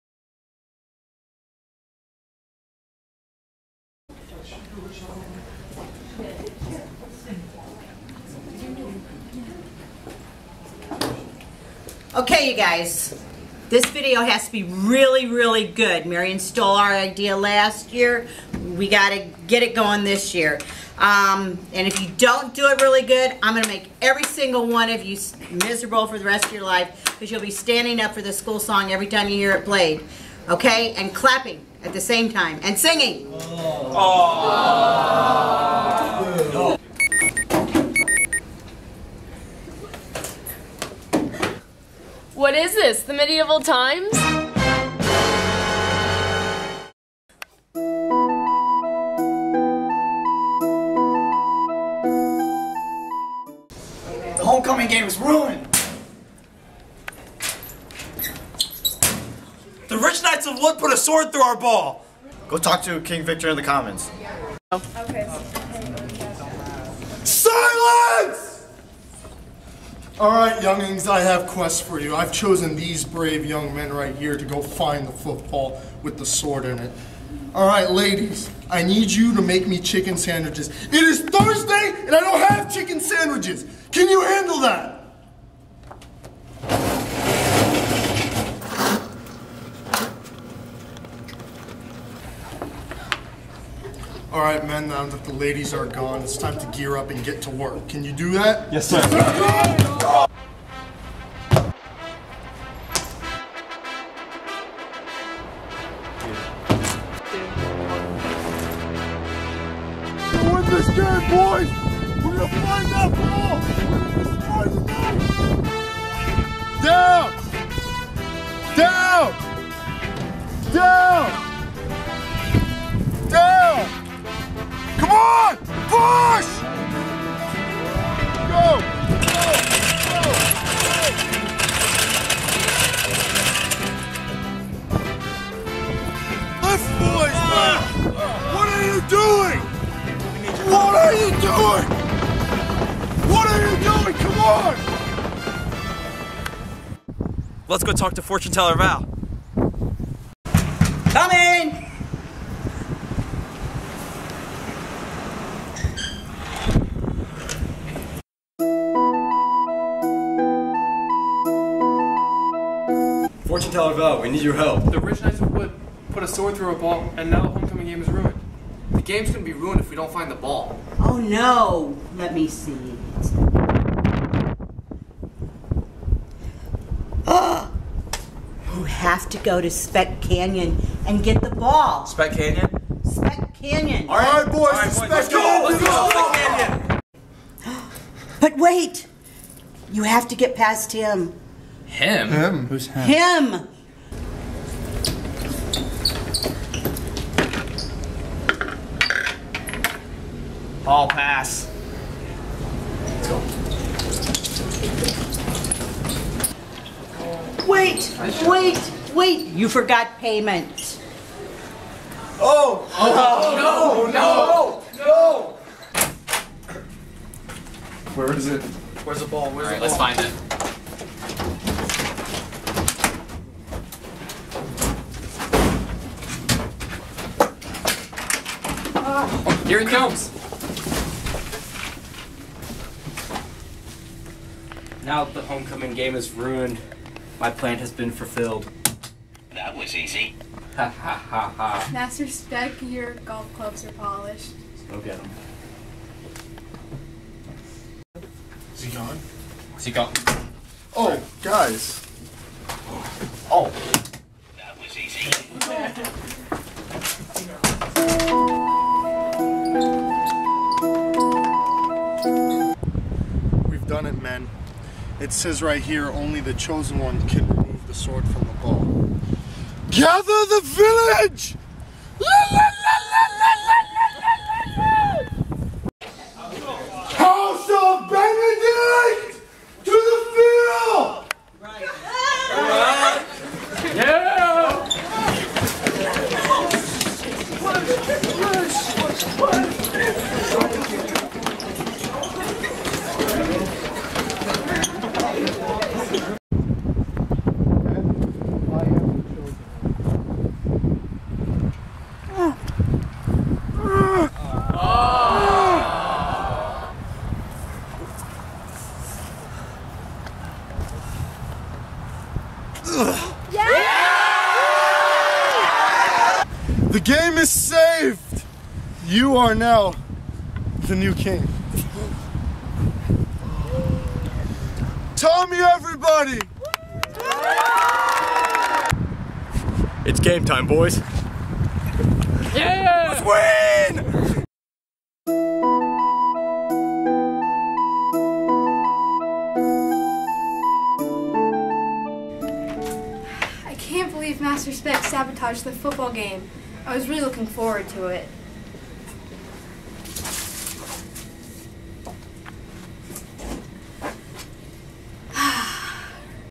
Benedict House Video 2019
Winner of the 2019 House Video Competition. (Tied with Carondelet)